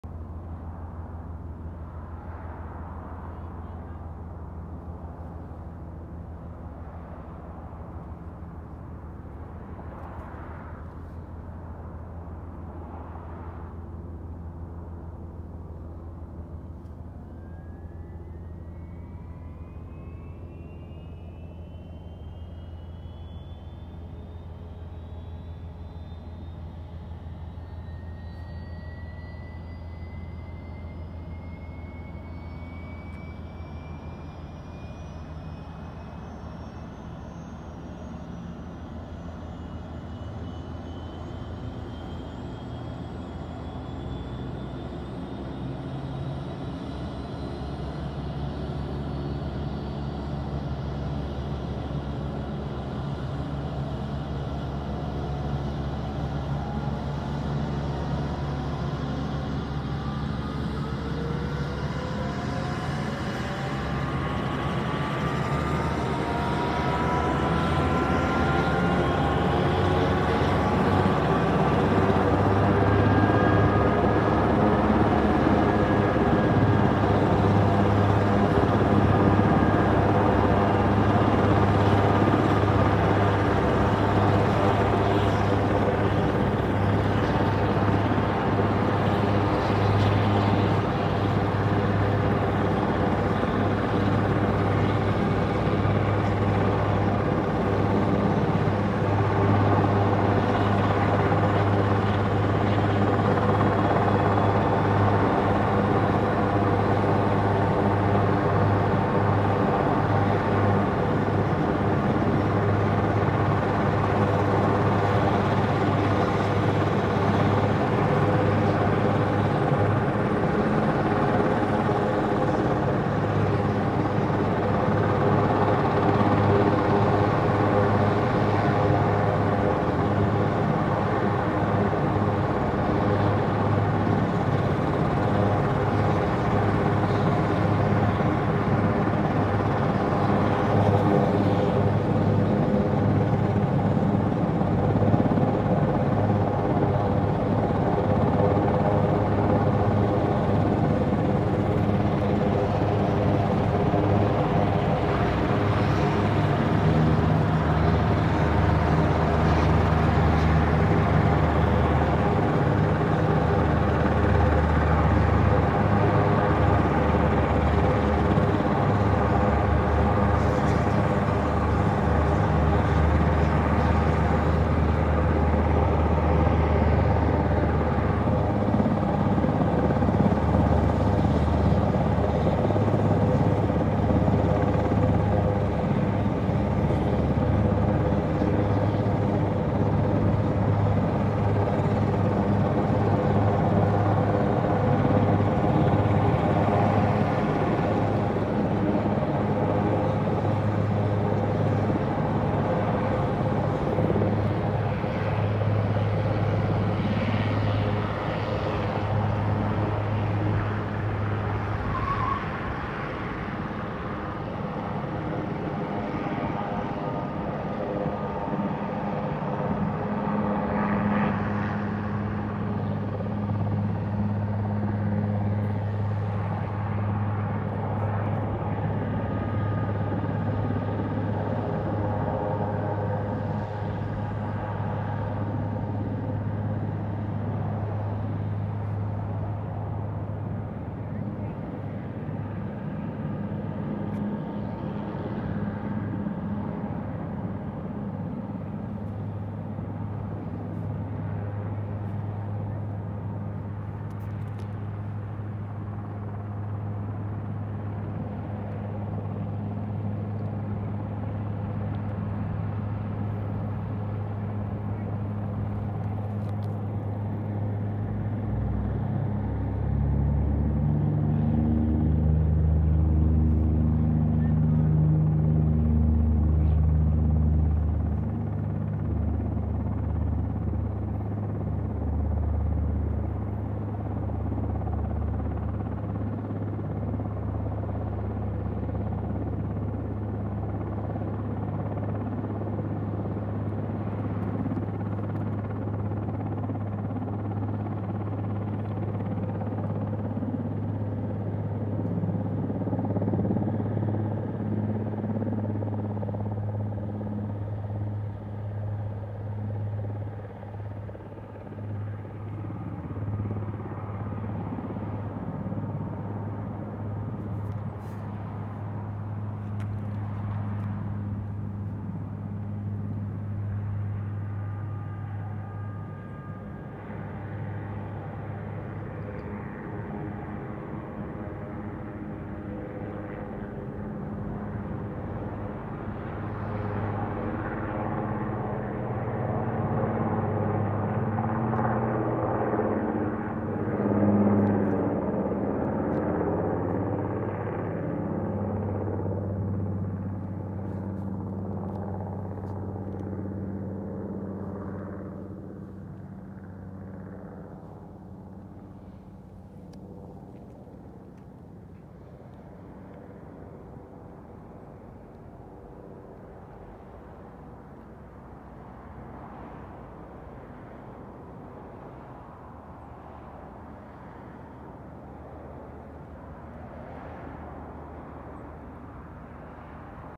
Los Angeles la county sheriffs department super puma rescue 5 n951lb
My shot of sheriffs 5 departing brackett airport